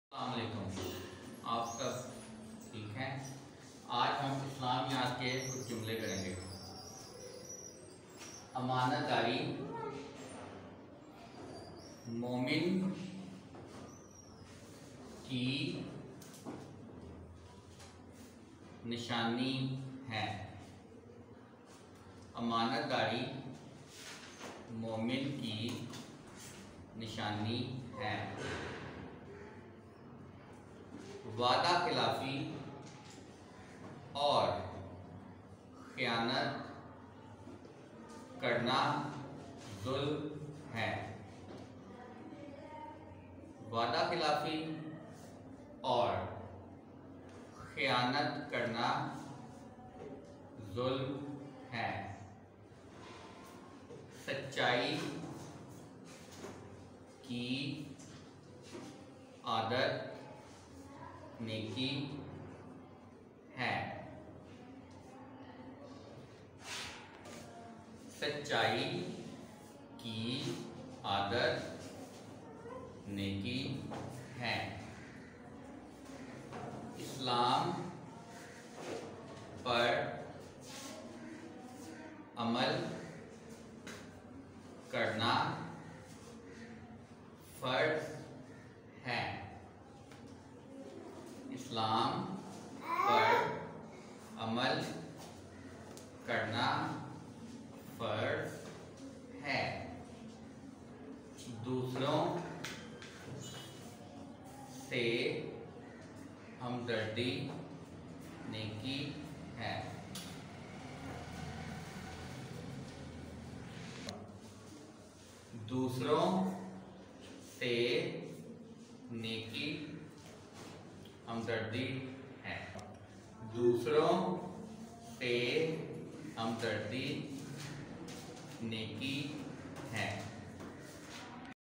learn how to sign islamic words in signlanguage
in this video i will teach you how to sign different   words related to islam.
in this video i wll teach sentences for convert then in signlanguage for understand our deaf community peoples.
this is vry easy way to understand and we can talk them after learn it simple sentences in sign language for people
in this vide we will learn how to talk with deaf peoples. there are 25 words lesson 3 . where we can learn these 25 words to talk with our deaf brothers sisters with sign language.
we try our best to  signs the words in thier orignal signs and according to the logics.
its our duty to make our self so much able to communicate with our deaf community rip.